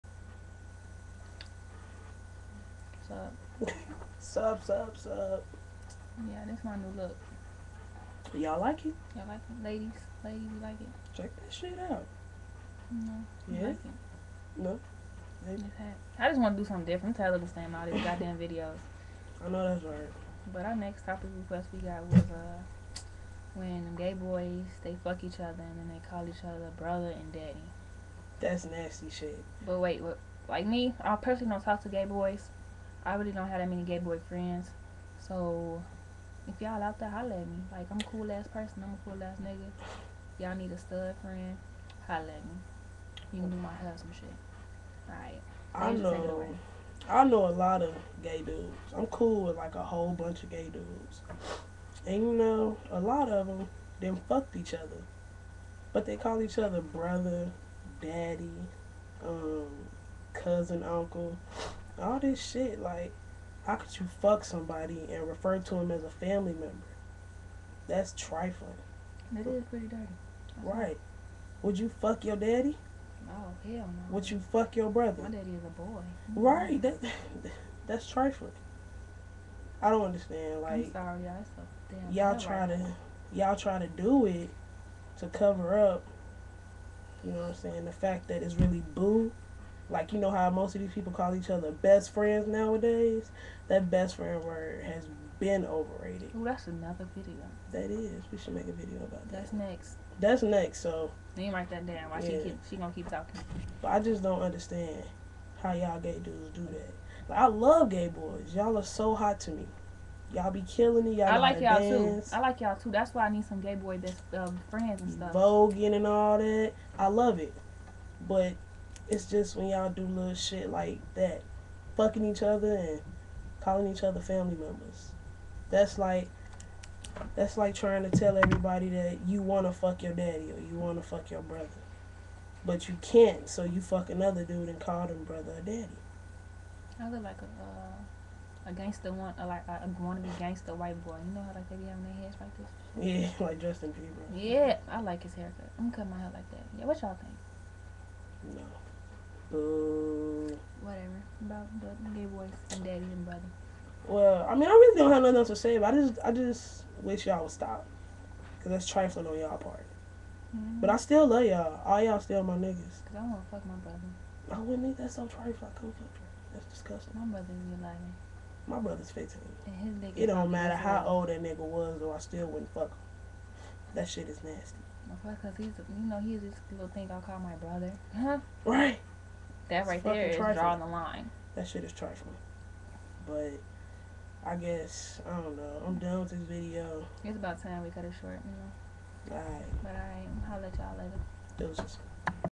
GAY BOYSZ FCKING/FAMILY MEMBERS with Gabby and Dasia
expressinq how we feel about qay dudes fuckinq each otha then callinq each otha brutha and daddy